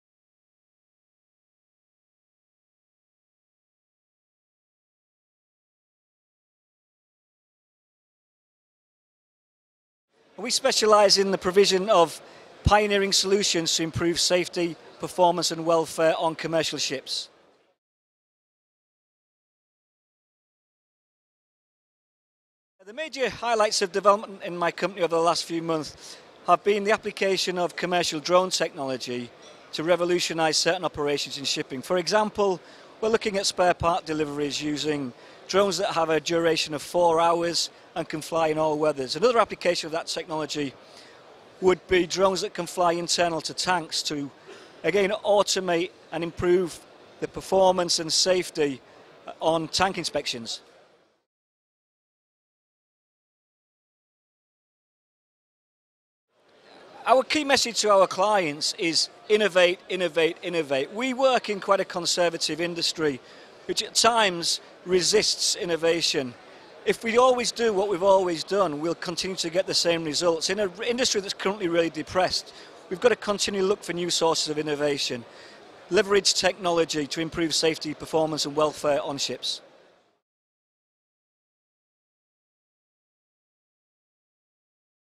Interview with Paul Luen, Martek Marine
SAFETY4SEA Interview with Paul Luen, CEO, Martek Marine at SMM 2016, September 6-9, Hamburg, Germany